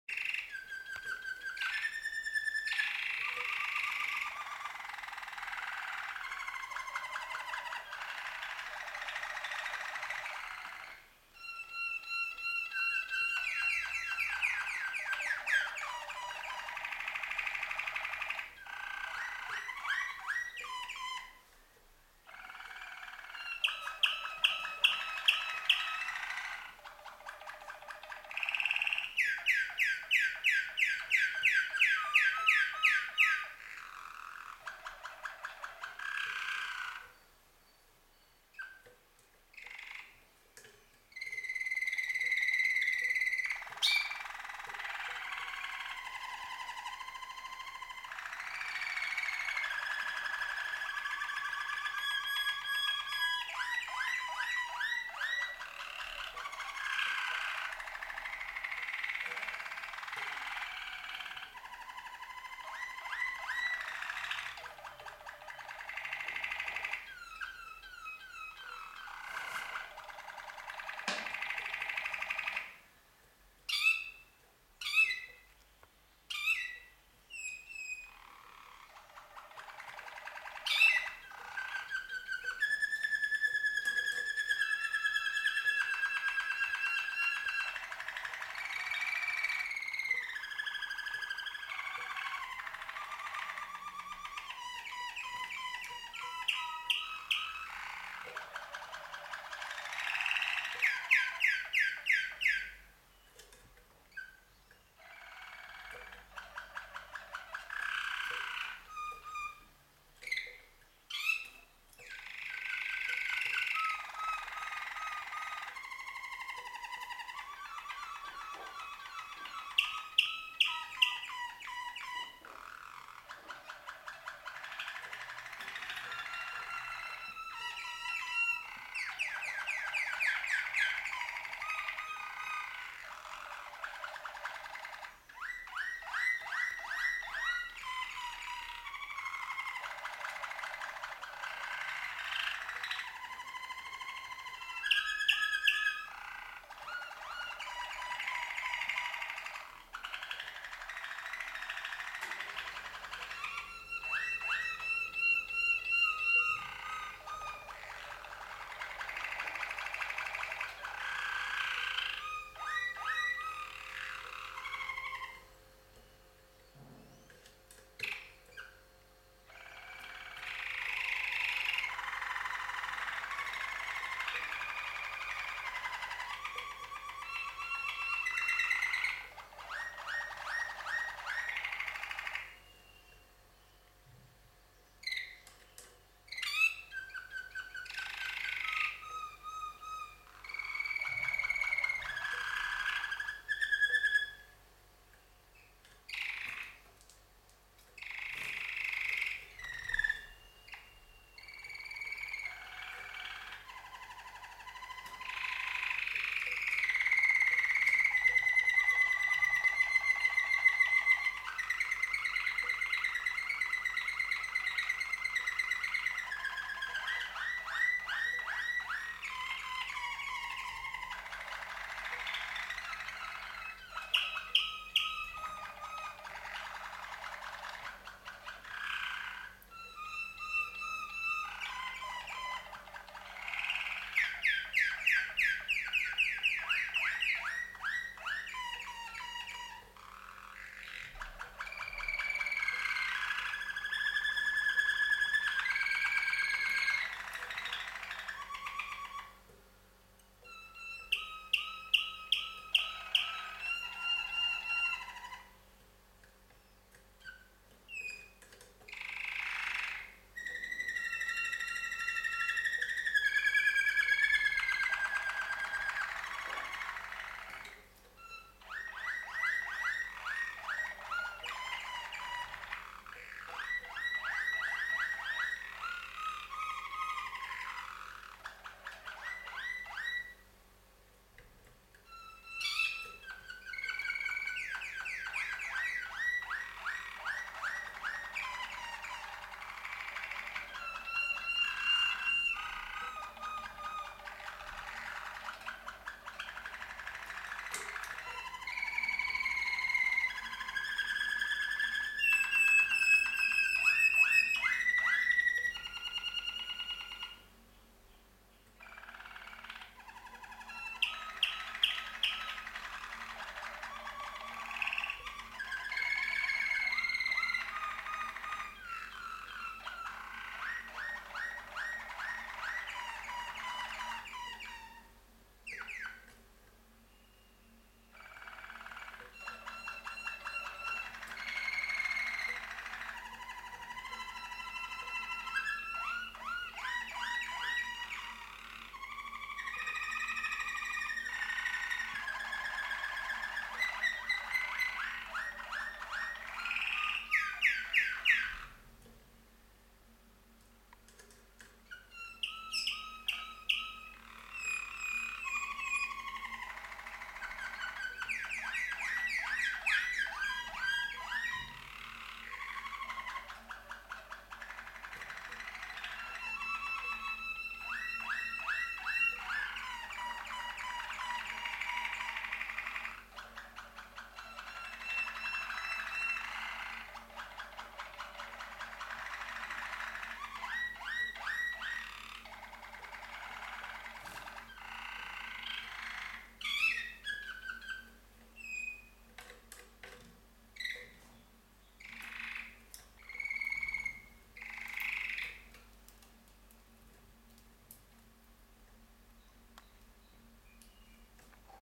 M2U01301
These are a dueto from Carduelis Major